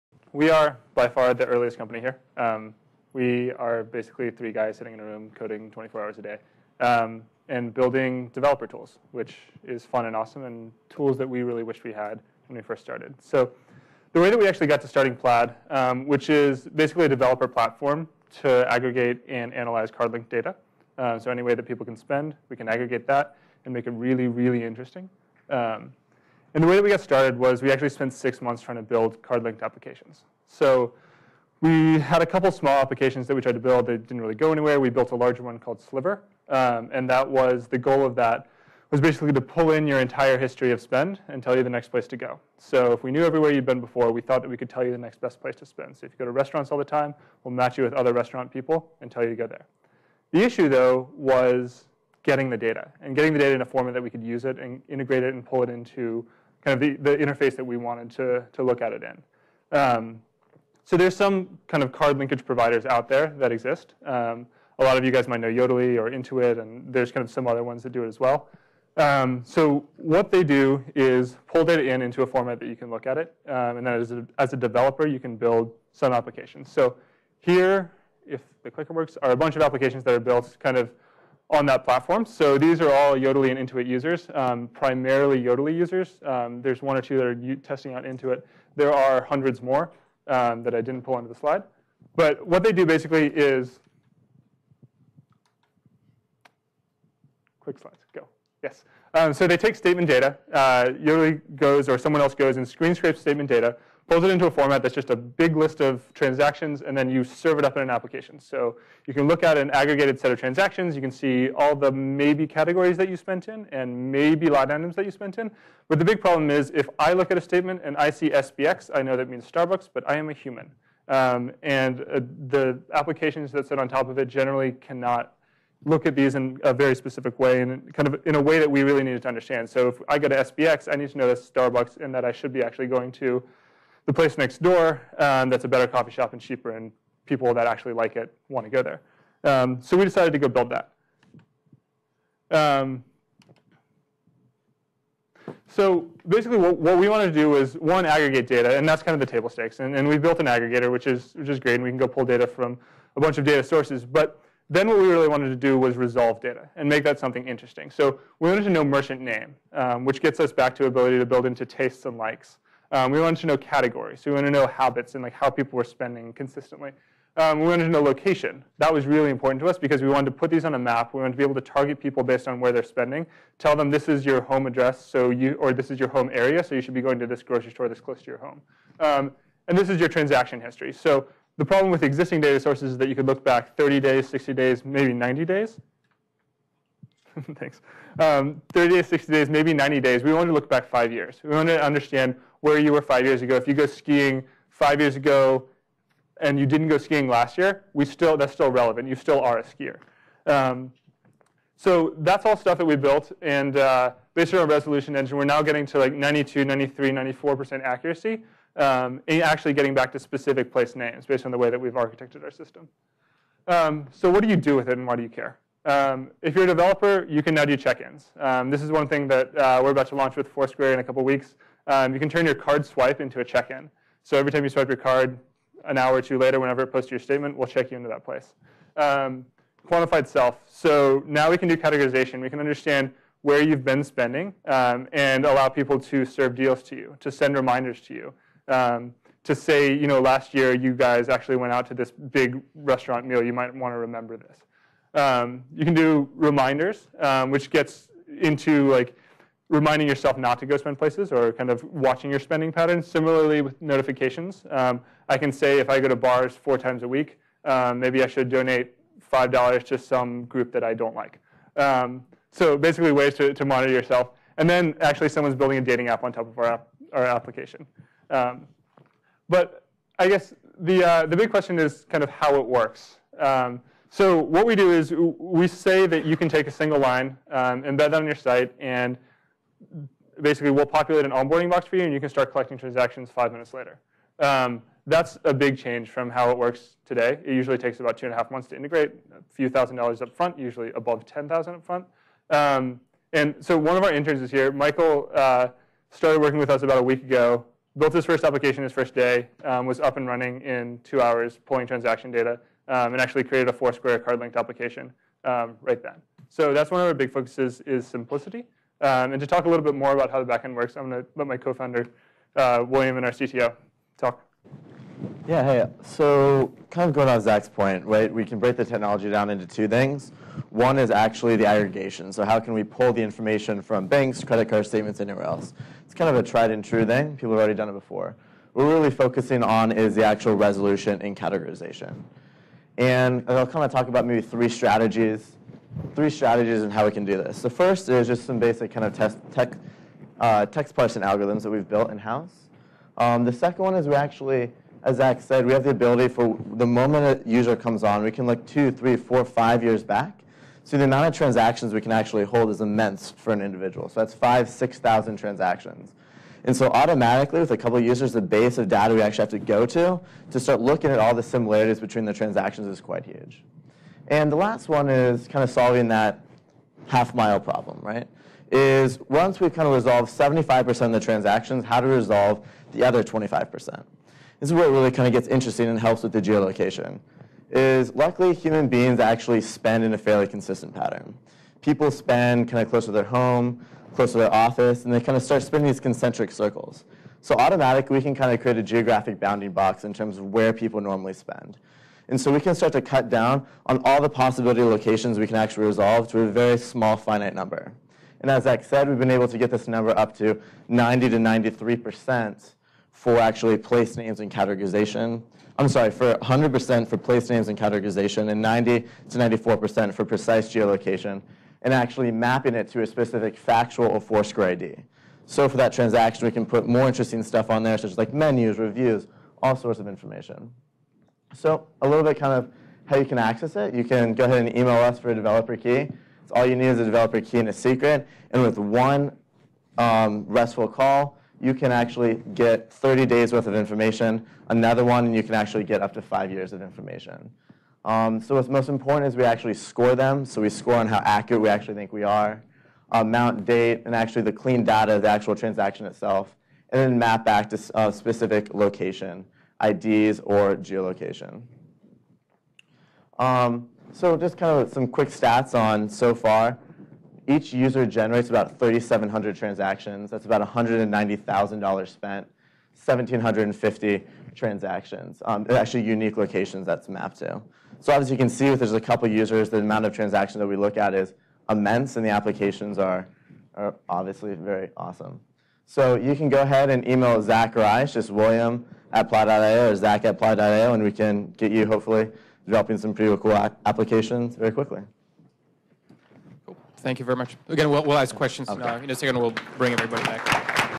Zach Perret and William Hockey, Plaid.io // NYC Data Business Meetup // Feb 2013
Zach Perret and William Hockey, co-founders of Plaid.io, present at the NYC Data Business Meetup on February 20, 2013 Find out more about Data Driven NYC